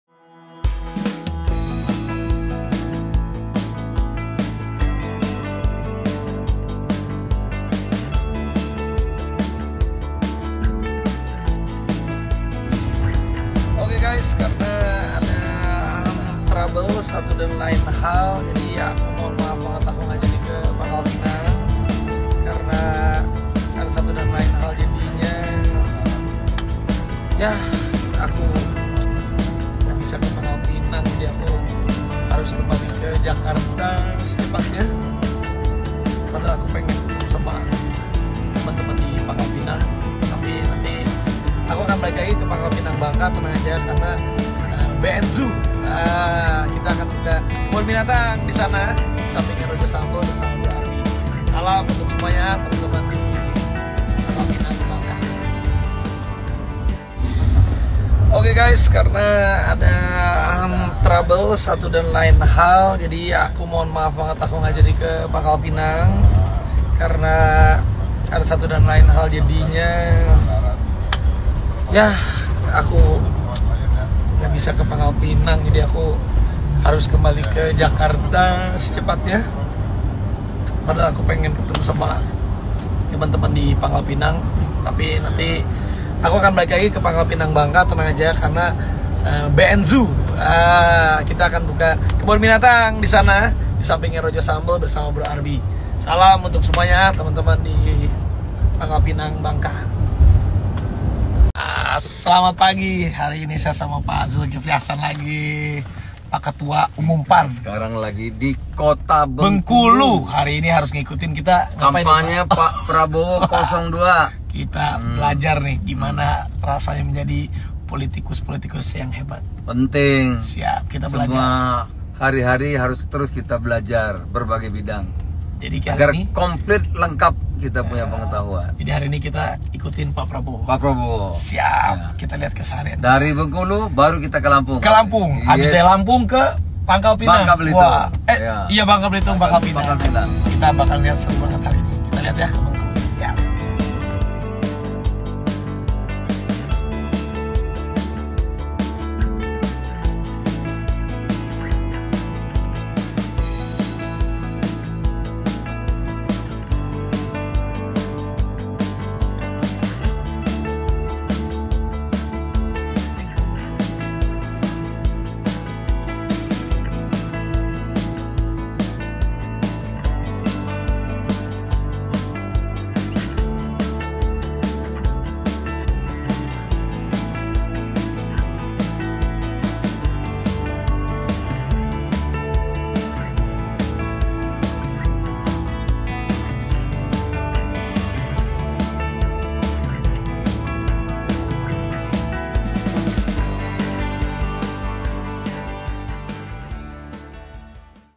Raffi Ahmad Batal Dampingi Prabowo Temui Relawan di Bangka Belitung
BANGKAPOS.COM - Prabowo Subianto, Calon Presiden (Capres) 02, dijadwalkan berkunjung ke Provinsi Bangka Belitung, Kamis (11/1/2023).

Prabowo dijadwalkan mendarat di Bandara Depati Amir, Pangkalpinang pukul 15.00 WIB. Pantauan Bangkapos.com di Gedung VVIP Bandara Depati Amir Pangkalpinang.

Terlihat di lokasi, sudah ada beberapa unit mobil mewah yang diketahui sudah terparkir sejak pukul 13.30 WIB.

Satu unit mobil Alphard warna putih dengan plat B 88 VM sudah terparkir di depan lobby gedung VVIP Bandara Depati Amir Pangkalpinang. Mobil tersebut diketahui yang akan membawa Menteri Pertahanan RI itu untuk mengikuti berbagai agenda di Provinsi Bangka Belitung.

Dari informasi yang dihimpun Bangkapos.com, agenda Prabowo di Babel mulanya akan didampingi oleh Ketua Umum PAN Zulkifli Hasan, Ketua Umum PBB Yusril Ihza Mahenda dan artis nasiona Raffi Ahmad.

Yusril Ihza Mahendra sendiri diketahui sudah tiba terlebih dahulu ke Pangkalpinang Bangka Belitung sekira pukul 11.00 WIB tadi.

Sekedar informasi, sebelum bertolak ke Babel, pada hari ini, Prabowo Sugianto juga melakukan kegiatan kampanye ke Bengkulu dan Bandar Lampung.

Namun diketahui, untuk agenda di Bangka Belitung tidak akan dihadiri oleh Raffi Ahmad.

“Raffi Ahmad batal, dari Lampung langsung terbang ke Jakarta,” ungkap salah seorang tim yang akan menyambut Prabowo di Gedung VVIP Bandara Depati Amir Pangkalpinang.

Hal tersebut juga terlihat di akun resmi milik Raffi Ahmad @raffinagita1717

“Oke guys, karena ada trouble satu dan lain hal. Aku mohon maaf banget enggak jadi ke Pangkalpinang karena ada satu dan lain hal jadinya yah aku enggak bisa ke Pangkalpinang, jadi aku harus kembali ke Jakarta secepatnya. Padahal aku pengen ketemu sama teman-teman di Pangkapinang, tapi nanti aku akan balik lagi ke Pangkapinang Bangka. Tenang aja, kita akan buka kebun binatang di sana, di samping Rojo Sambel,” ucap Raffi dalam instagram storynya.

Hingga pukul 15.30 WIB, sampai saat ini Prabowo Subianto belum kunjung tiba di Bangka Belitung.

(Bangkapos.com/Arya Bima Mahendra)

VP: Tomi